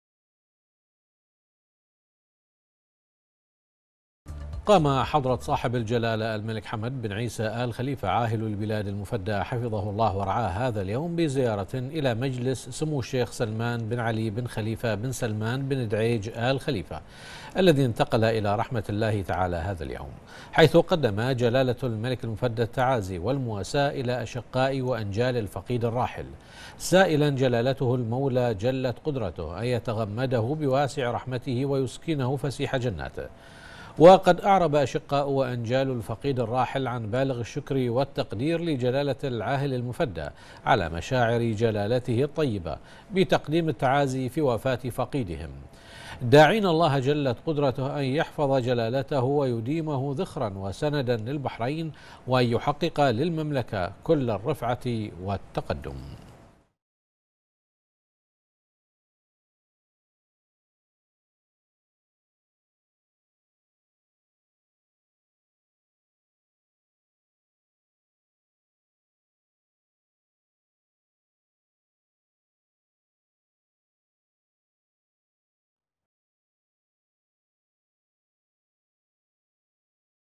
البحرين: عاهل البلاد المفدى يقدم التعازي والمواساة إلى أشقاء وأنجال سمو الشيخ سلمان بن علي بن خليفة
المنامة في 26 أغسطس / بنا / قام حضرة صاحب الجلالة الملك حمد بن عيسى آل خليفة عاهل البلاد المفدى حفظه الله ورعاه هذا اليوم بزيارة الى مجلس سمو الشيخ سلمان بن علي بن خليفة بن سلمان بن دعيج آل خليفة الذي انتقل الى رحمة الله تعالى هذا اليوم، حيث قدم جلالة الملك المفدى التعازي والمواساة الى اشقاء وانجال الفقيد الراحل، سائلا جلالته المولى جلت قدرته أن يتغمده بواسع رحمته ويسكنه فسيح جناته.